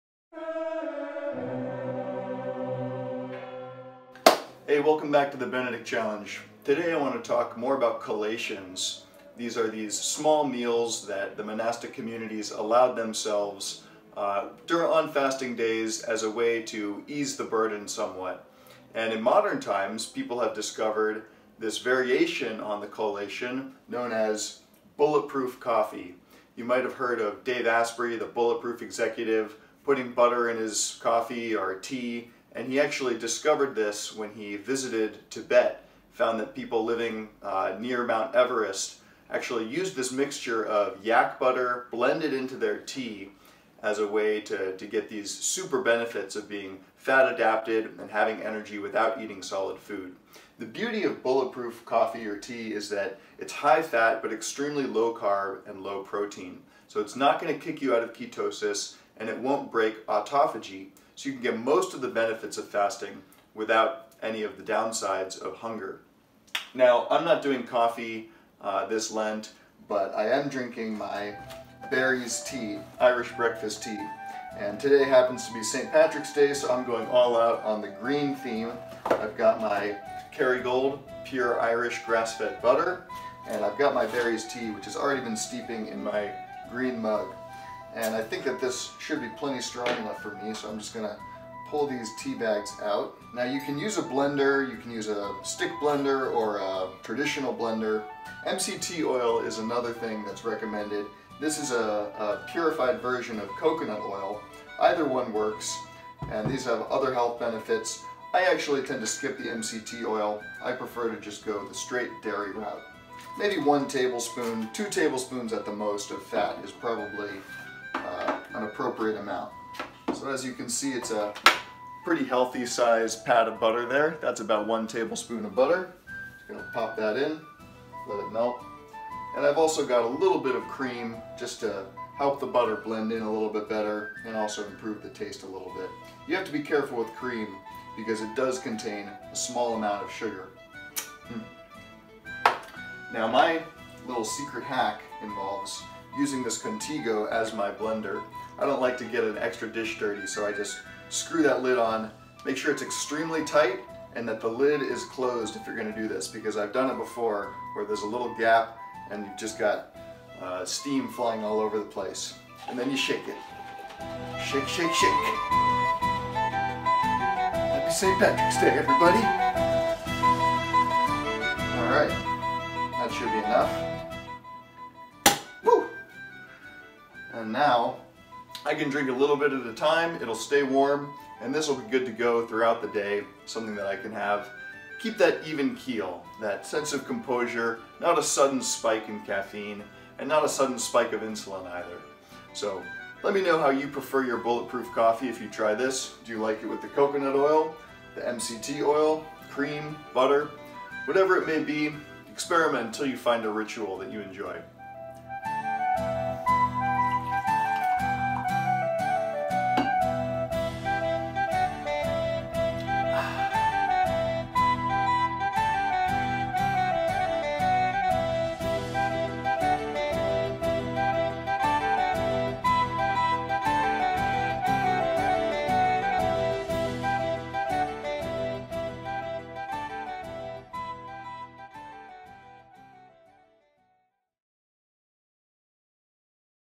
Make Bulletproof Coffee (Without Making a Mess)
This video continues my series *The Benedict Challenge: Creating a Keepable Rhythm of Spiritual Fasting* to help us weakened, modern people return to the vital ancient tradition of the church.
Intermittent fasting is all the rage, and bulletproof coffee was the original lifehack for getting most of the benefits, such as ketosis and autophagy, without experiencing hunger, low energy, or weakness.

The problem is that, like most lifehacks, it can feel like more of a hassle than it's worth. That's where this neat little trick comes in - just shake vigorously in a thermos and you have smooth, creamy, butter coffee without making a mess.
ANaturalMethod.com/benedict